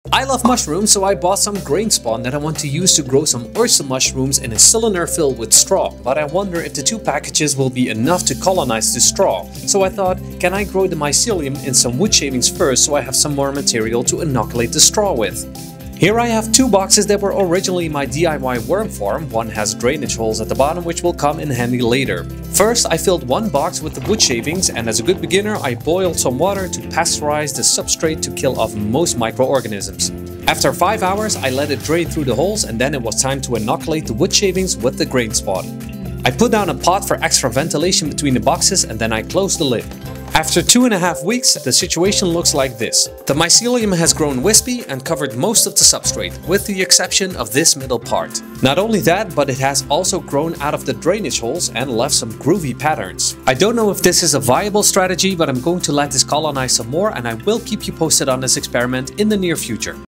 🍄 The OYSTER MUSHROOM Experiment!
As a beginner when it comes to growing my own edible mushrooms, I'm trying out this experiment to increase the amount of oyster mushroom mycelium that I can use to inoculate some straw, much like feeding your sourdough starter. So far I seem to be on the right path, but I will keep you posted on the progress!